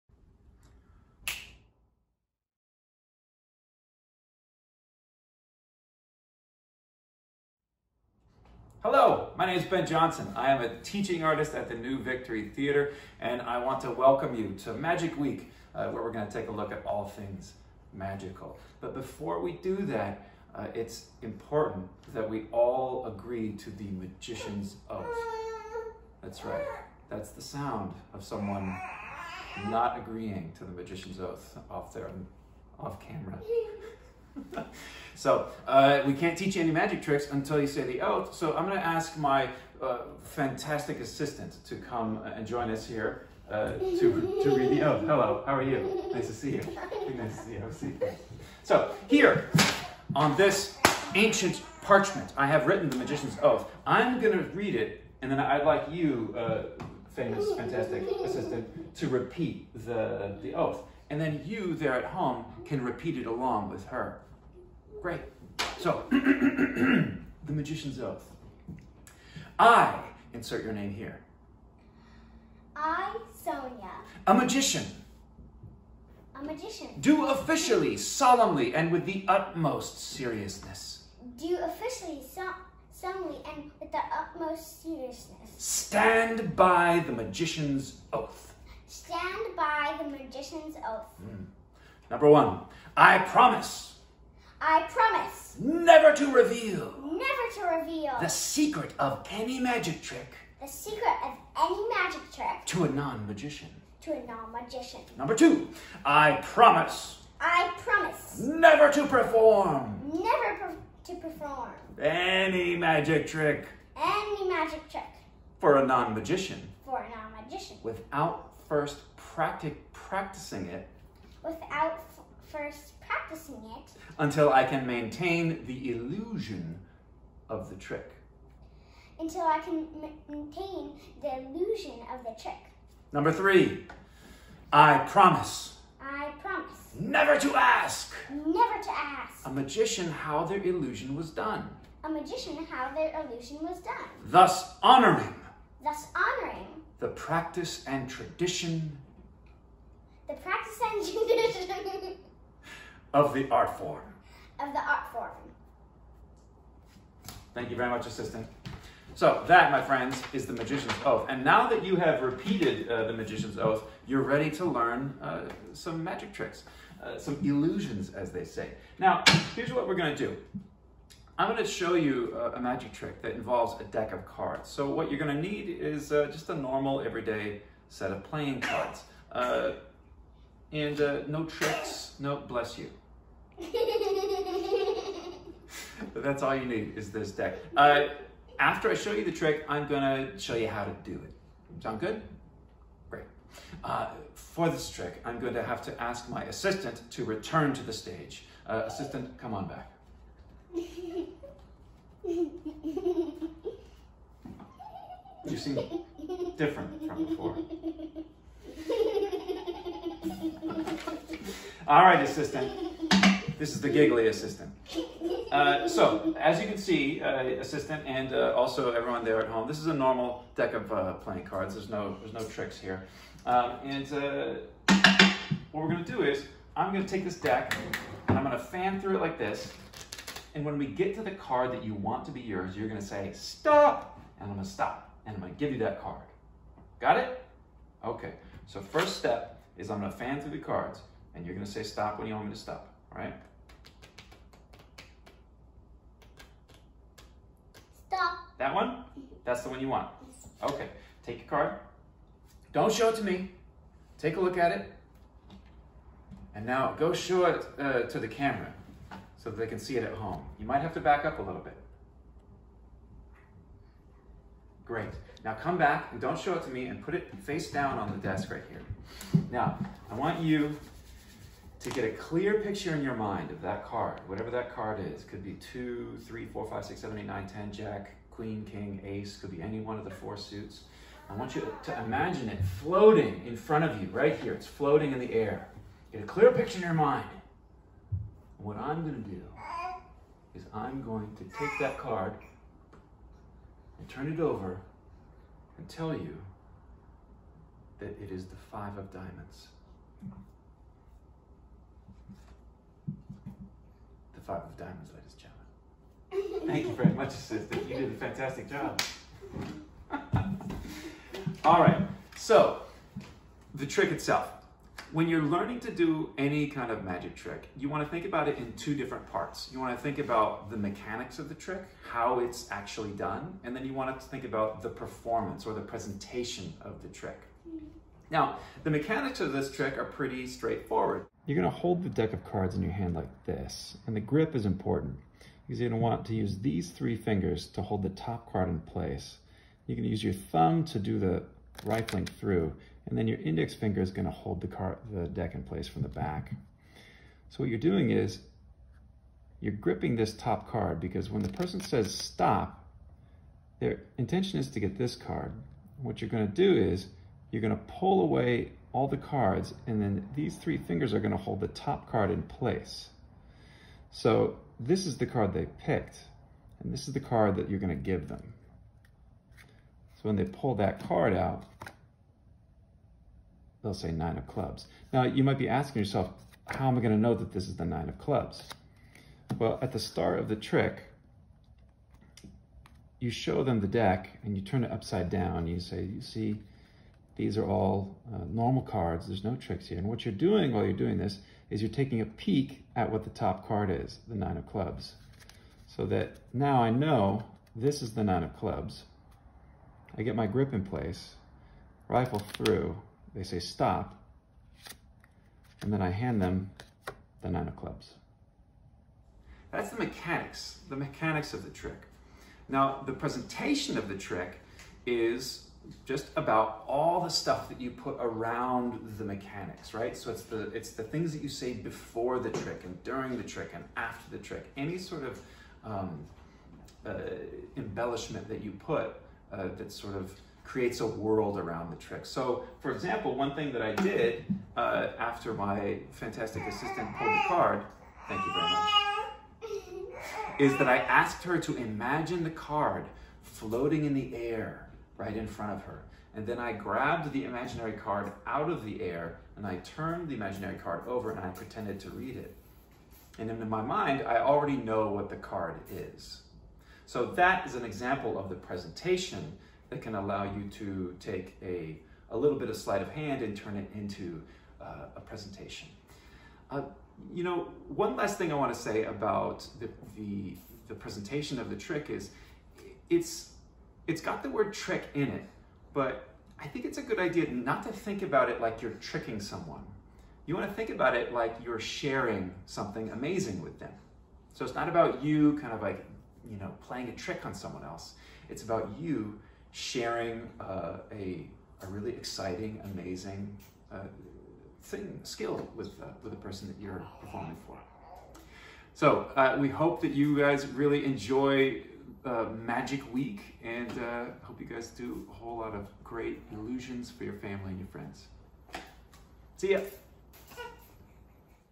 New Victory Arts Break – Magic Week – Monday
Follow along with New Victory Teaching Artist Ben Johnson as he leads his assistant through the Magician's Oath and then teaches an amazing card trick!